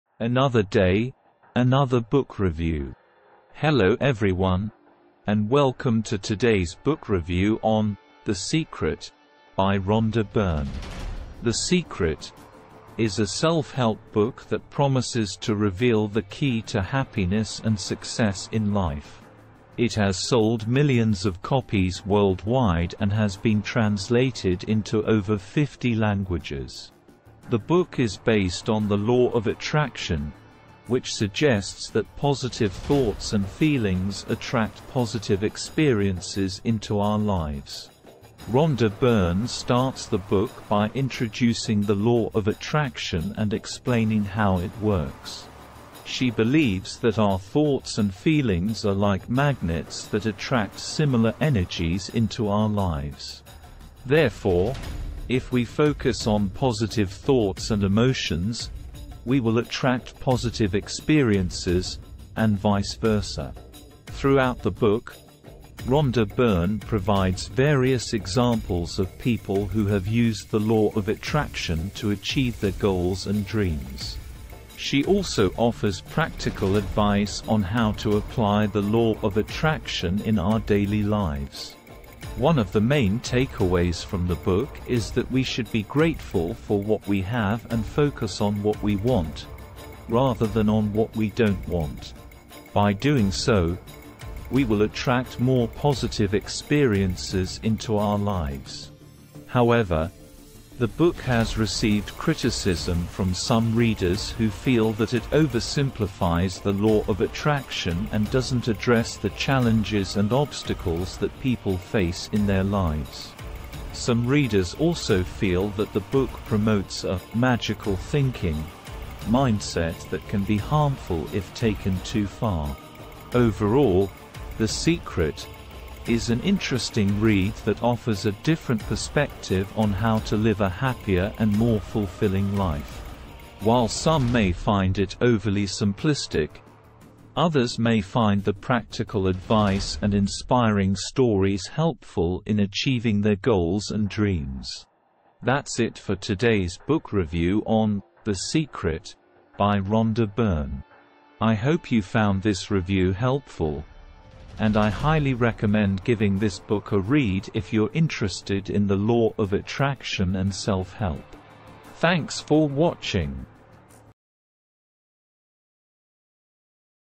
“The Secret” By Rhonda Byrne | Book Review
"The Secret" is an interesting read that offers a different perspective on how to live a happier and more fulfilling life. While some may find it overly simplistic, others may find the practical advice and inspiring stories helpful in achieving their goals and dreams.